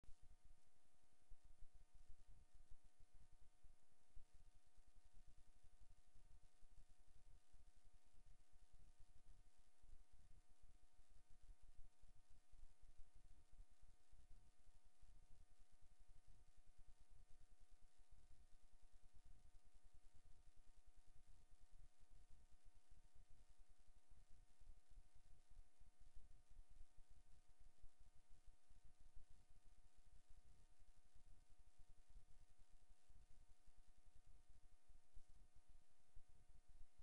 Hassel Island kitchen exterior model
This model was created in PhotoModeler by Reid Horton from digital images collected in the Virgin Islands National Park in March 2012 under a cooperative agreement between the University of Maine and the National Park Service.